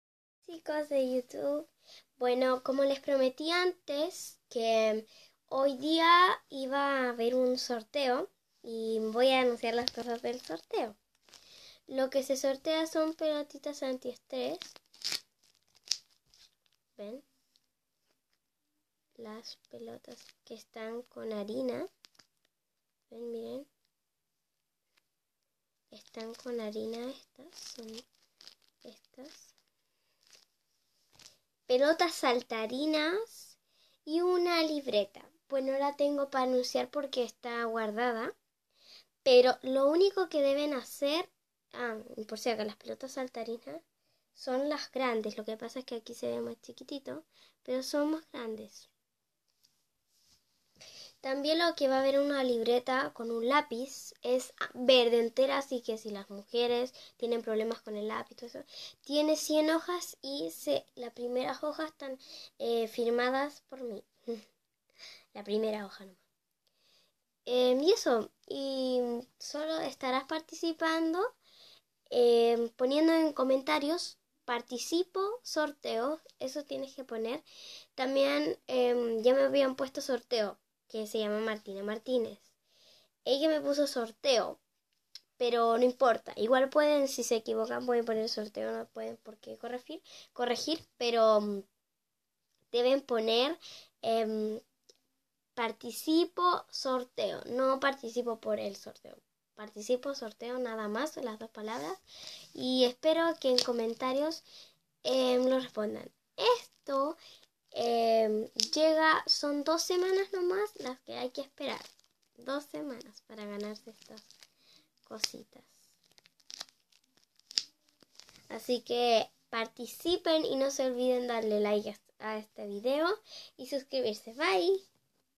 Sorteo y premios!!
Los premios son :
Una pelotita antiestres
Pelotas saltarinas gigantes
Una libreta y un lapiz...recuerda poner en comentarios participo sorteo...y ya estaras participando